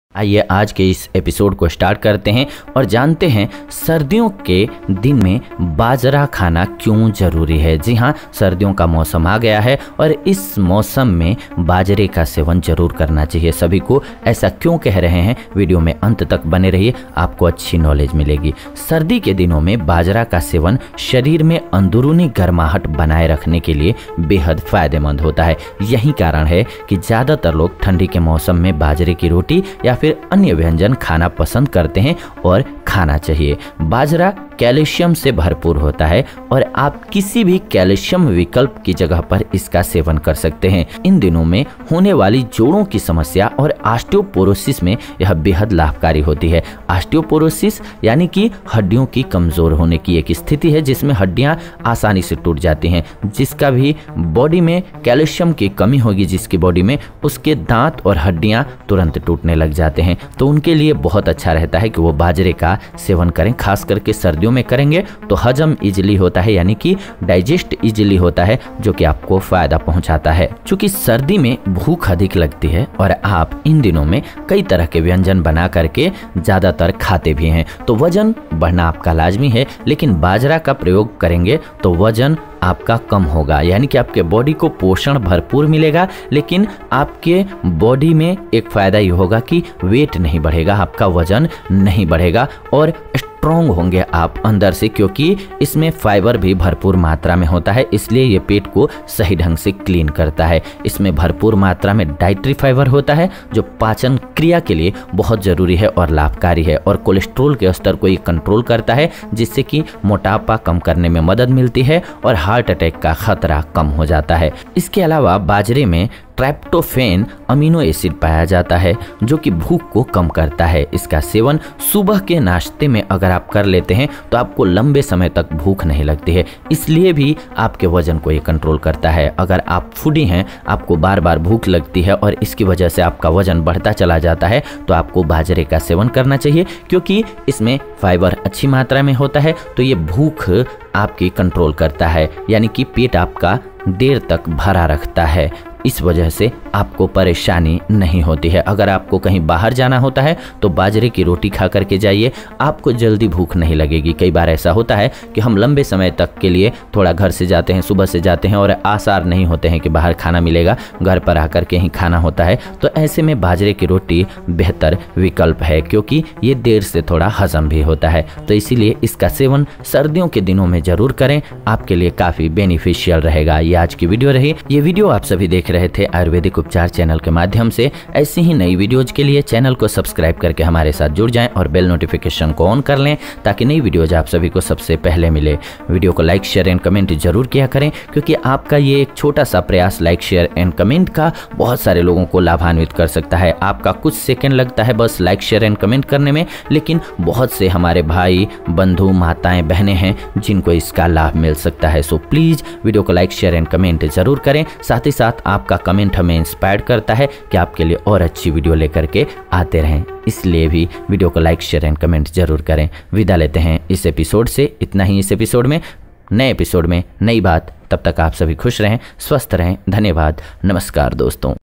सर्दियों में क्यों जरुरी है बाजरे का सेवन करना!! Bajre Ke Fayde | Health Benefits of Bajra
Aayurvedik upchar#

गर्भाशय, बच्चेदानी में गांठ या रसौली होने का कारण, लक्षण, इलाज, उपचार, दवा, Fibroid ka ilaj in hindi